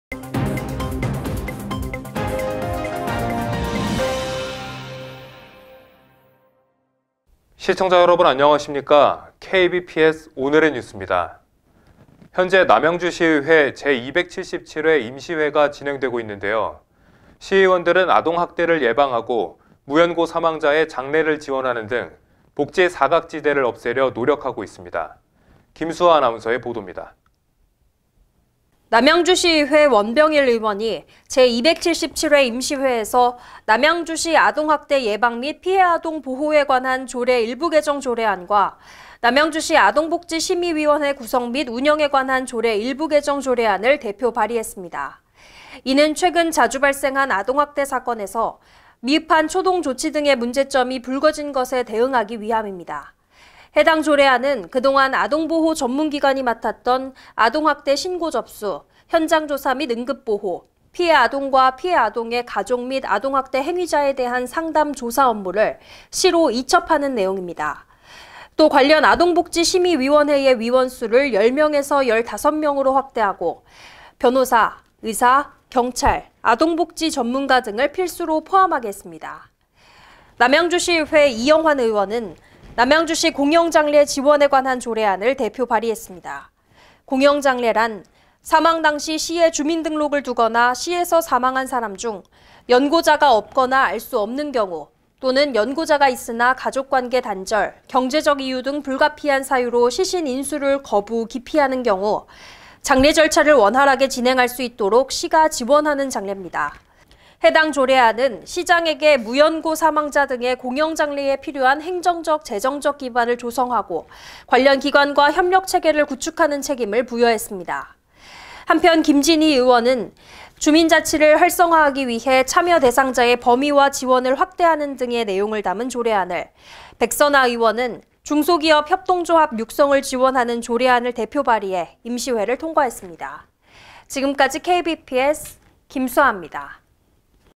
남양주시의회, 아동학대방지 및 공영장례 지원으로 복지사각지대 해소 노력
남양주시의회(의장 이철영) 원병일 의원이 제277회 임시회에서‘남양주시 아동학대 예방 및 피해아동 보호에 관한 조례 일부개정조례안’과‘남양주시 아동복지심의위원회 구성 및 운영에 관한 조례 일부개정조례안’을 대표 발의했다. 이는 최근 자주 발생한 아동학대사건에서 미흡한 초동 조치 등의 문제점이 붉어진 것에 대응하기 위함이다. 해당 조례안은 그동안 아동보호 전문기관이 맡았던 아동학대 신고 접수, 현장조사 및 응급보호, 피해아동과 피해아동의 가족 및 아동학대행위자에 대한 상담·조사 업무를 시로 이첩하는 내용이다. 또, 관련 아동복지심의위원회의 위원수를 10명에서 15명으로 확대하고 변호사, 의사, 경찰, 아동복지 전문가 등을 필수로 포함하게 했다.
 남양주시의회 이영환 의원은 ‘남양주시 공영장례 지원에 관한 조례안’을 대표 발의했다. ‘공영장례’란 사망 당시 시에 주민등록을 두거나 시에서 사망한 사람 중 연고자가 없거나 알 수 없는 경우 또는 연고자가 있으나 가족관계 단절, 경제적 이유 등 불가피한 사유로 시신 인수를 거부·기피하는 경우 장례절차를 원활하게 진행할 수 있도록 남양주시가 지원하는 장례이다. 해당 조례안은 시장에게 무연고 사망자 등의 공영장례에 필요한 행정적·재정적 기반을 조성하고 관련 기관과 협력체계를 구축하는 책임을 부여했다.
 한편, 김진희 의원은 주민자치를 활성화하기 위해 참여 대상자의 범위와 지원을 확대하는 등의 내용을 담은 조례안을, 백선아 의원은 중소기업 협동조합 육성을 지원하는 조례안을 대표 발의해 임시회를 통과했다.